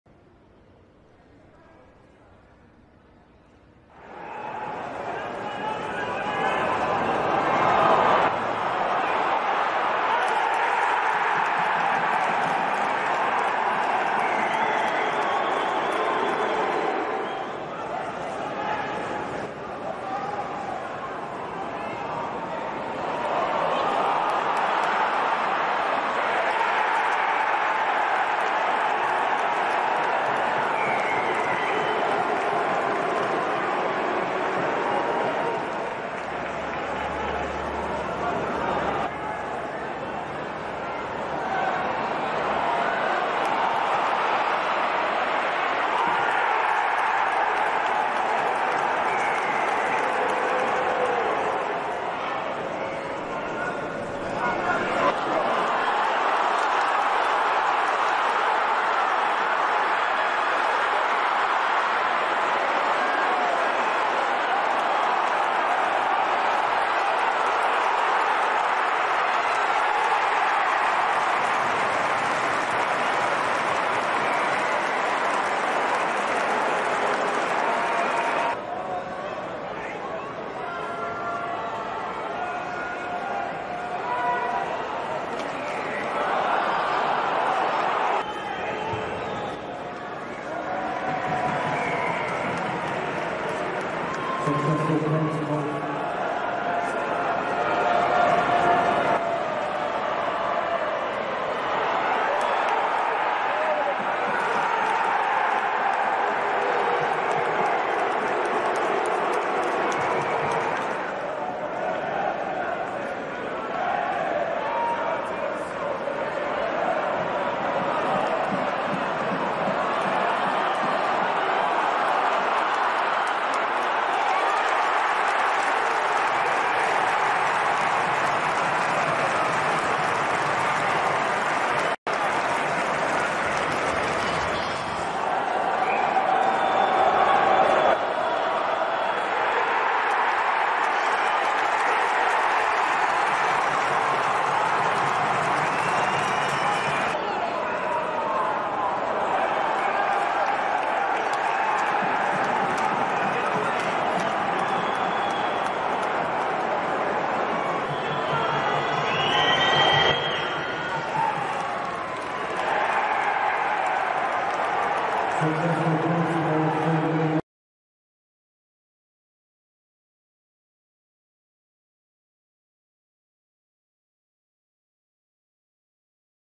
Highlights | Hull FC v Leigh Leopards, Round 6, 2023 Betfred Super League
Hull FC face Leigh Leopards as Round 6 of 2023 Betfred SuperLeague concludes.